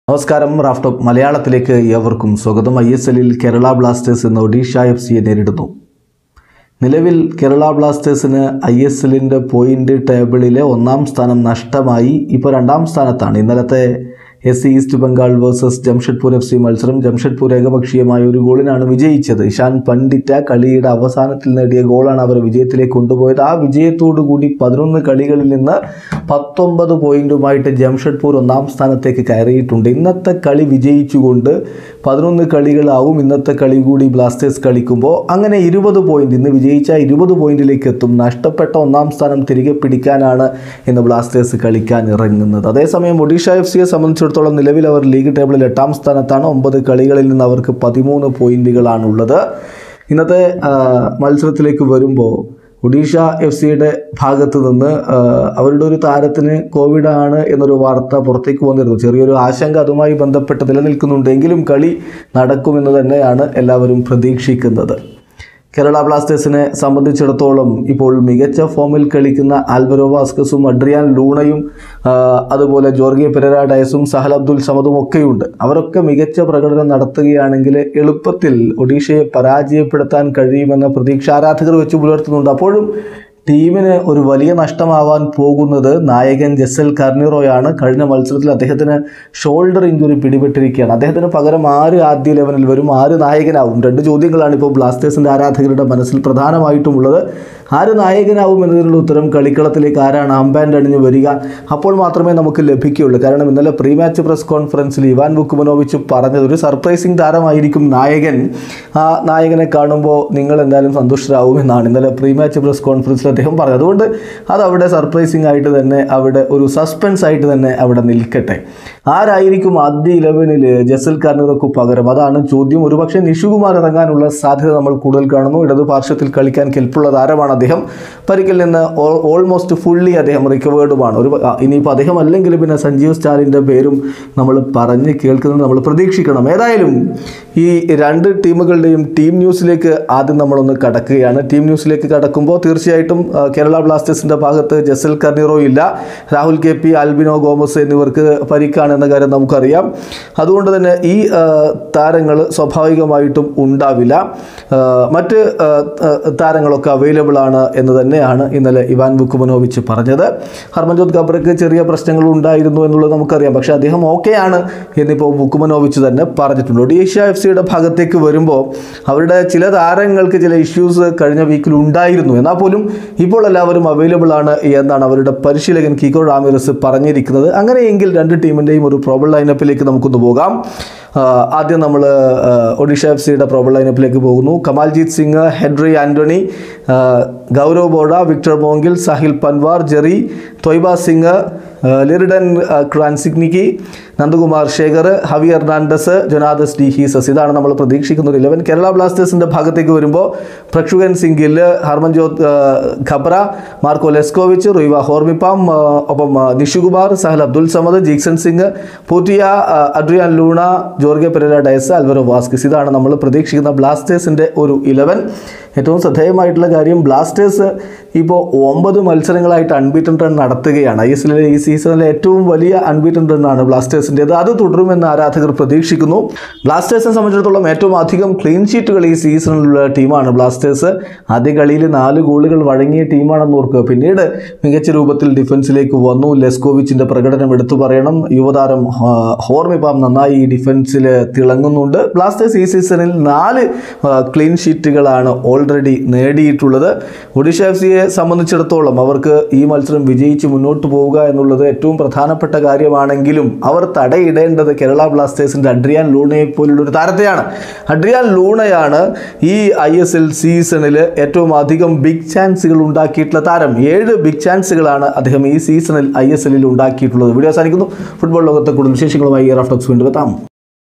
ഒഡീഷയെ ഒരിക്കൽ കൂടി തകർക്കണം, ഒന്നാം സ്ഥാനം തിരികെ പിടിക്കണം: ബ്ലാസ്റ്റേഴ്സ് റെഡി |OFC vs KBFC
Odisha FC vs Kerala Blasters FC
ISL